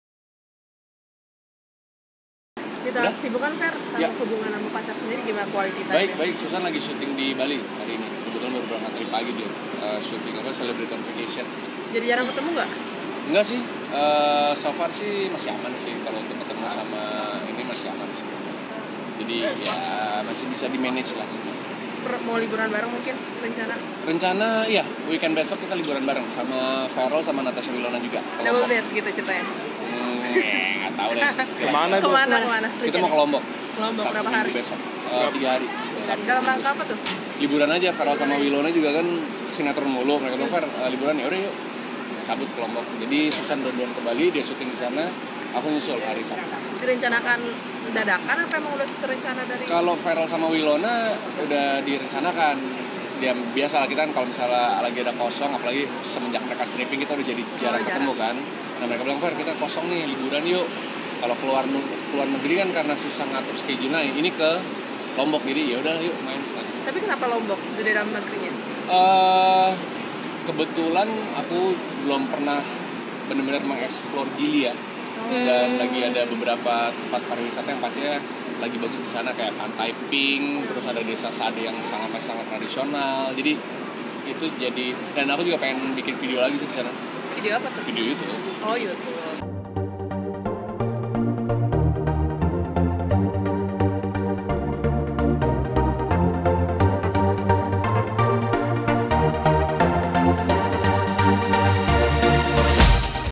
Double Date! Fero Walandow dan Susan Sameh Bakal Liburan Bareng Verell dan Natasha Wilona
Grid.ID - Double Date! Fero Walandow dan Susan Sameh Bakal Liburan Bareng Verell dan Natasha Wilona